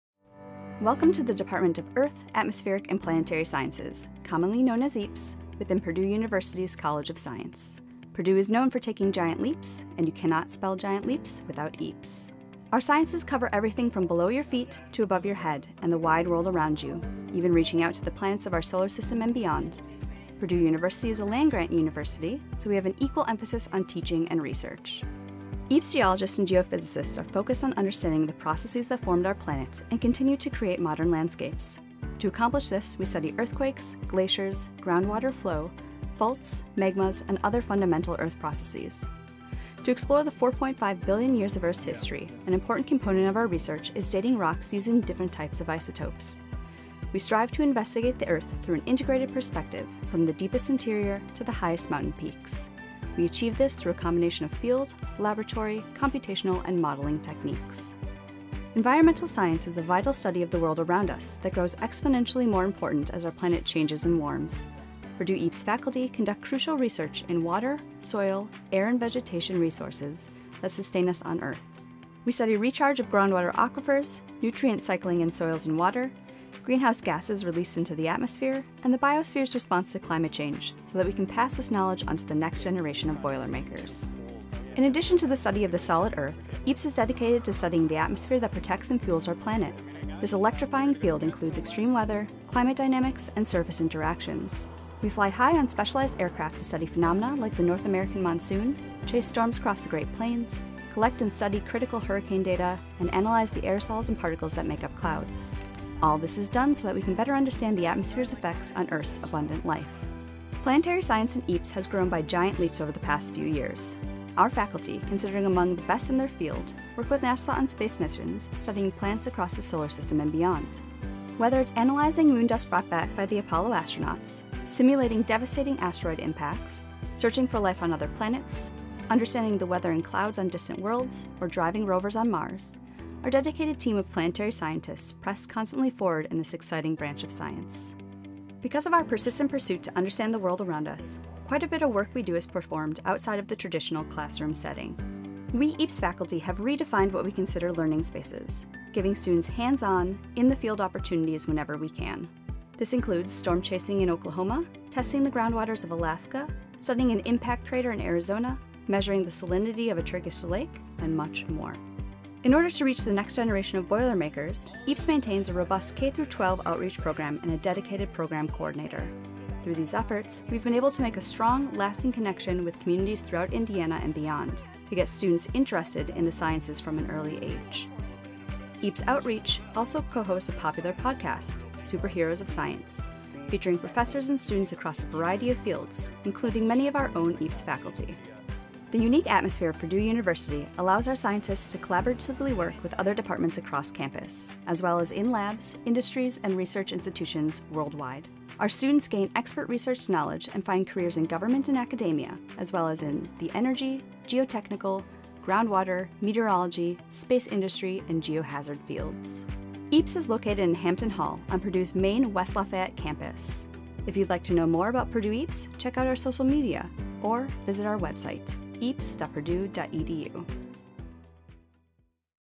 Purdue University Department of Earth, Atmospheric, and Planetary Sciences (EAPS)
Welcome to the Purdue University College of Science’s Department of Earth, Atmospheric, and Planetary Sciences, commonly known as EAPS.  Purdue is known for taking Giant Leaps, and you cannot spell Giant Leaps without EAPS.
Our sciences cover everything below your feet, and the wide world around you, even reaching out to distant planets.  Purdue University is a land grant university, so we have an equal emphasis on teaching and research.
Learn more about our department at eaps.purdue.edu
Video produced by Cole Crider (Purdue University College of Science)
Photos and video provided by Purdue University, Purdue EAPS faculty, and NASA.